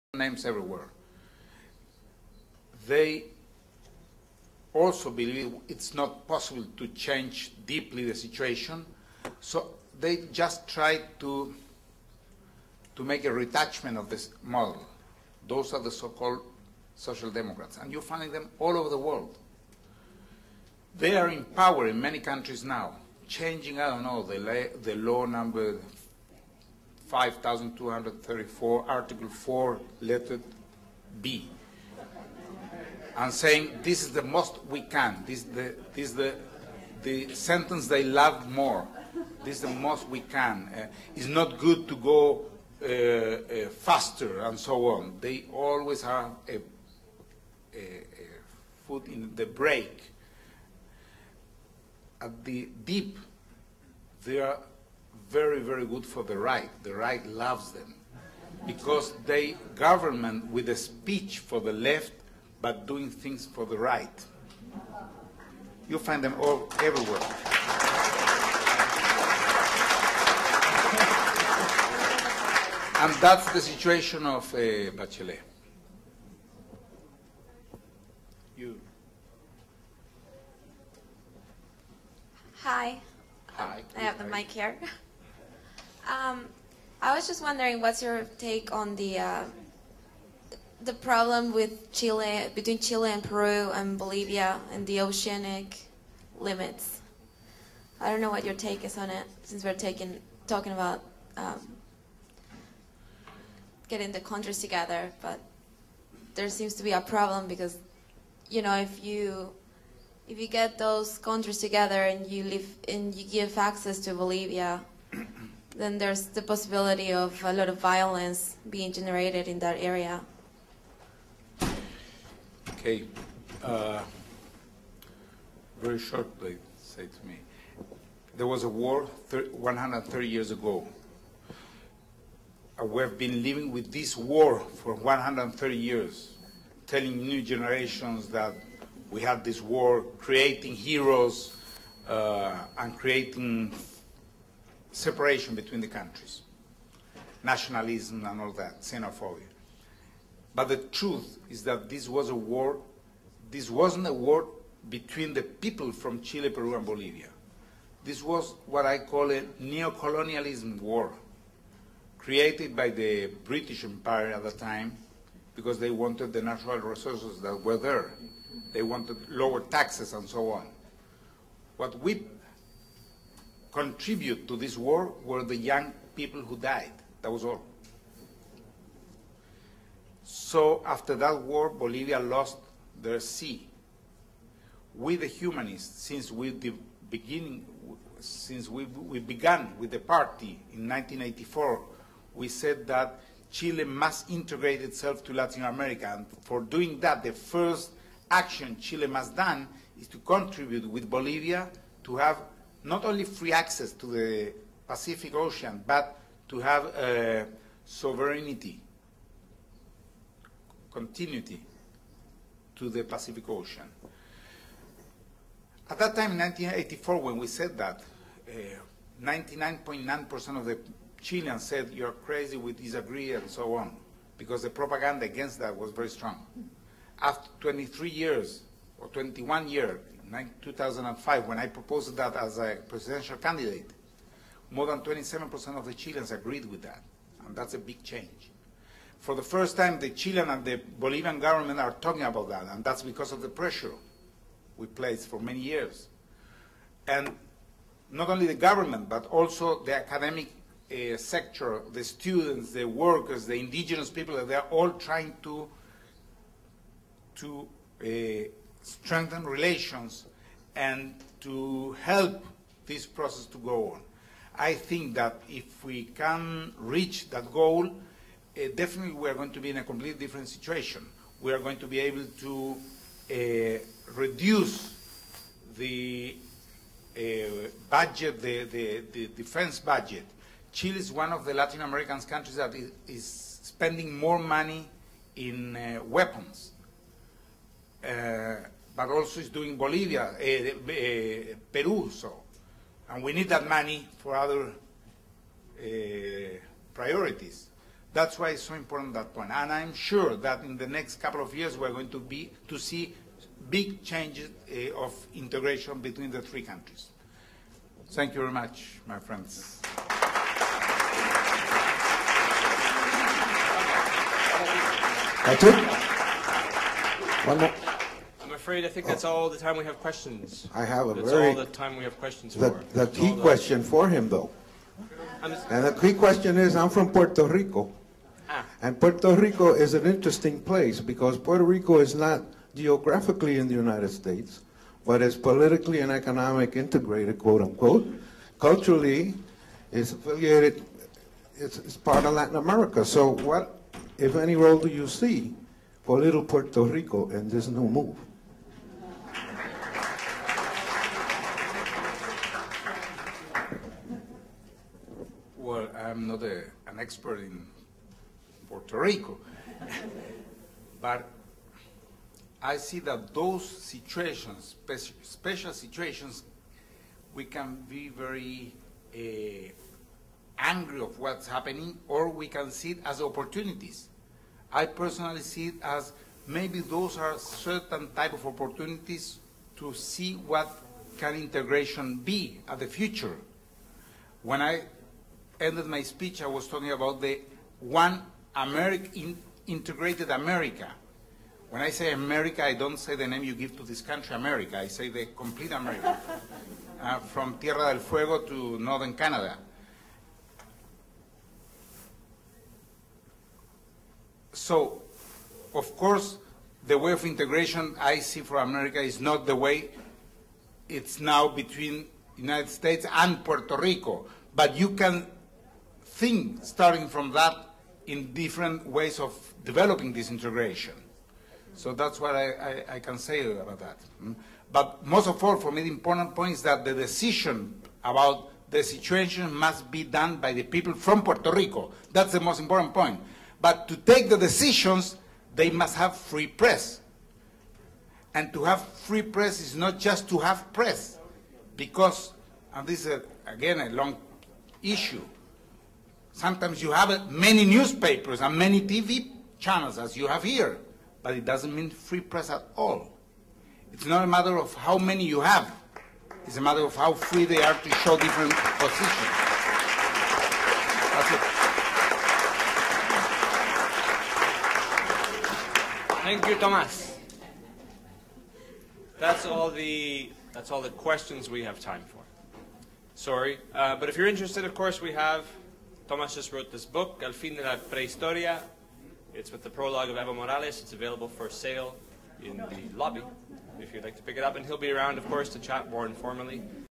Tomas Hirsch Nov 17th - 6 of 6
Tomas Hirsch - North American New Humanist Forum - Hunter College School of Social Works - November 17th 2007